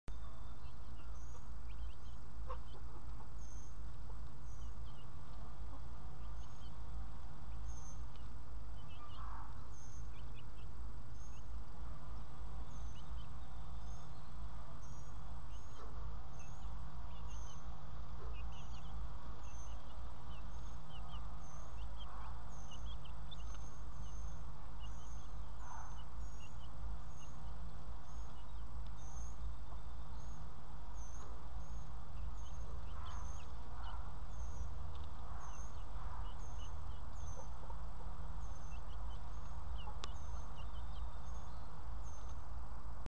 Silkie chicks eating some Curly Dock 5
Curly Dock has essential vitamins and minerals that help aid the Silkie chicks in their growth and maturation